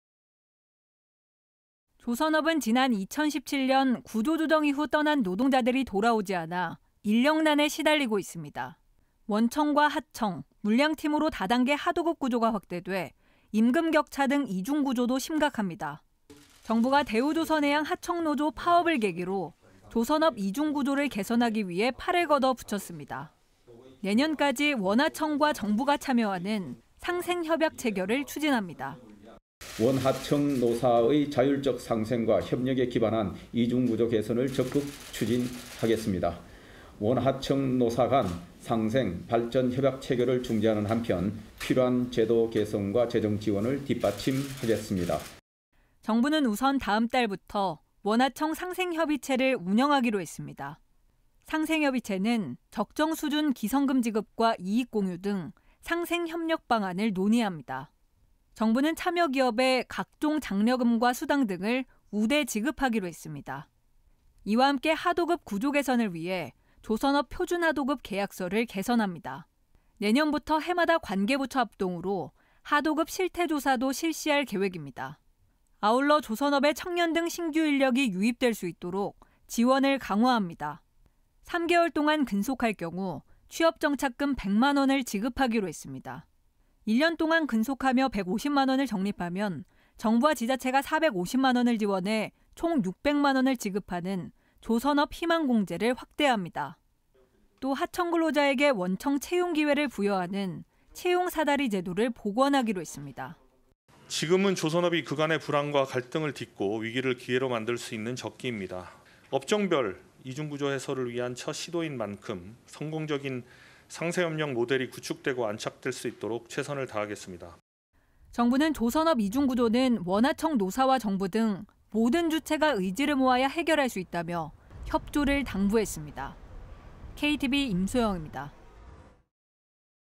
조선업 상생협의체 운영…원하청 이중구조 개선
정부가
비상경제장관
회의를 열고
조선업의
원청과 하청
임금격차 등을
개선하기 위한
대책을
마련했습니다.

원하청
상생협의체 통해
사회적 대화로
해법을
마련하도록
지원하기로
했습니다.